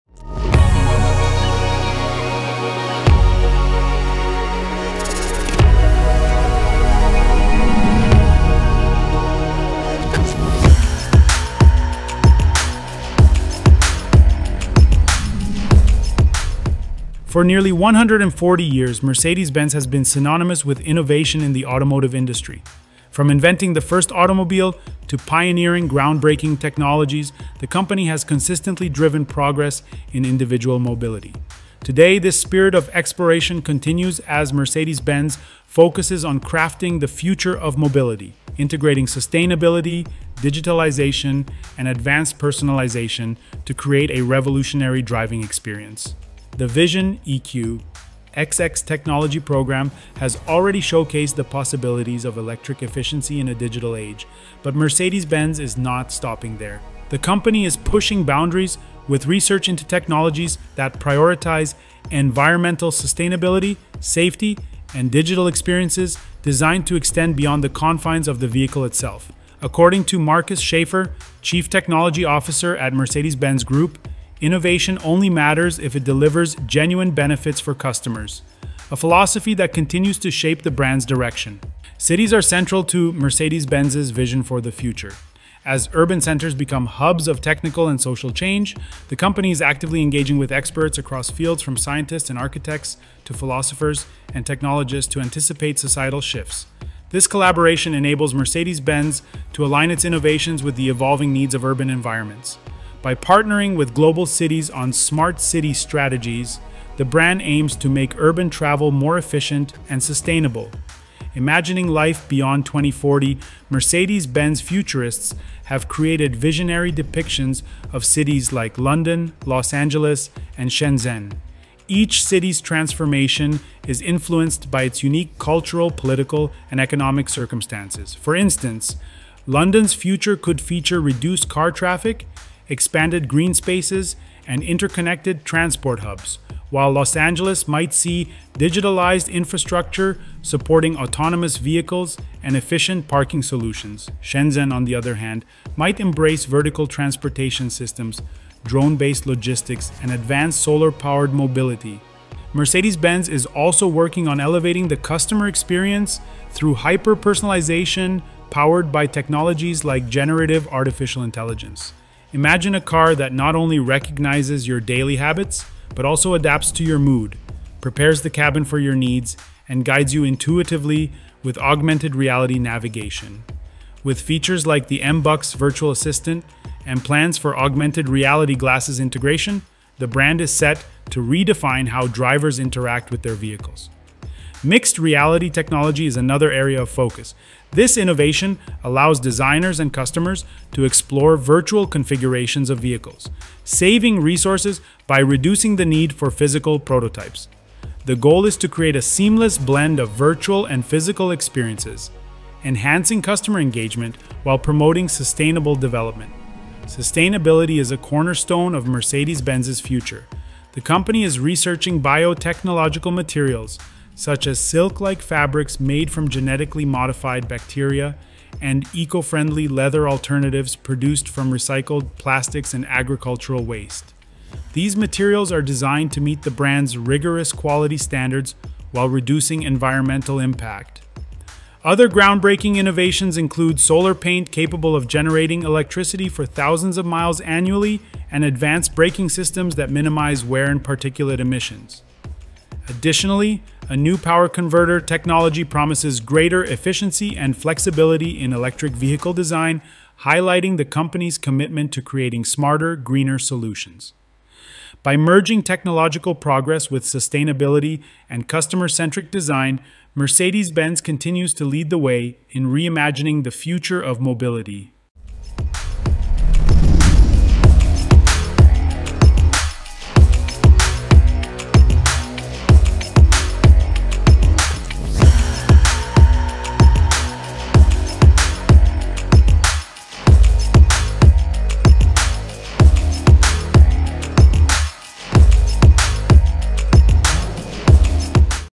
Mercedes Provides Exclusive Future Technologies
For nearly one hundred and forty years, Mercedes-Benz has been synonymous with innovation in the automotive industry. From inventing the first automobile to pioneering groundbreaking technologies, the company has consistently driven progress in individual mobility. Today, this spirit of exploration continues as Mercedes-Benz focuses on crafting the future of mobility, integrating sustainability, digitalization, and advanced personalization to create a revolutionary driving experience.

The VISION EQ XX technology program has already showcased the possibilities of electric efficiency in a digital age, but Mercedes-Benz is not stopping there. The company is pushing boundaries with research into technologies that prioritize environmental sustainability, safety, and digital experiences designed to extend beyond the confines of the vehicle itself. According to Markus Schäfer, Chief Technology Officer at Mercedes-Benz Group, innovation only matters if it delivers genuine benefits for customers, a philosophy that continues to shape the brand's direction.

Cities are central to Mercedes-Benz’s vision for the future. As urban centers become hubs of technical and social change, the company is actively engaging with experts across fields from scientists and architects to philosophers and technologists to anticipate societal shifts. This collaboration enables Mercedes-Benz to align its innovations with the evolving needs of urban environments. By partnering with global cities on "smart city" strategies, the brand aims to make urban travel more efficient and sustainable.

Imagining life beyond twenty forty, Mercedes-Benz futurists have created visionary depictions of cities like London, Los Angeles, and Shenzhen. Each city’s transformation is influenced by its unique cultural, political, and economic circumstances. For instance, London’s future could feature reduced car traffic, expanded green spaces, and interconnected transport hubs, while Los Angeles might see digitalized infrastructure supporting autonomous vehicles and efficient parking solutions. Shenzhen, on the other hand, might embrace vertical transportation systems, drone-based logistics, and advanced solar-powered mobility.

Mercedes-Benz is also working on elevating the customer experience through hyper-personalization powered by technologies like generative artificial intelligence. Imagine a car that not only recognizes your daily habits but also adapts to your mood, prepares the cabin for your needs, and guides you intuitively with augmented reality navigation. With features like the M BUX Virtual Assistant and plans for augmented reality glasses integration, the brand is set to redefine how drivers interact with their vehicles.

Mixed reality technology is another area of focus. This innovation allows designers and customers to explore virtual configurations of vehicles, saving resources by reducing the need for physical prototypes. The goal is to create a seamless blend of virtual and physical experiences, enhancing customer engagement while promoting sustainable development.

Sustainability is a cornerstone of Mercedes-Benz’s future. The company is researching biotechnological materials, such as silk-like fabrics made from genetically modified bacteria and eco-friendly leather alternatives produced from recycled plastics and agricultural waste. These materials are designed to meet the brand's rigorous quality standards while reducing environmental impact.

Other groundbreaking innovations include solar paint capable of generating electricity for thousands of miles annually and advanced braking systems that minimize wear and particulate emissions. Additionally, a new power converter technology promises greater efficiency and flexibility in electric vehicle design, highlighting the company’s commitment to creating smarter, greener solutions.

By merging technological progress with sustainability and customer-centric design, Mercedes-Benz continues to lead the way in reimagining the future of mobility.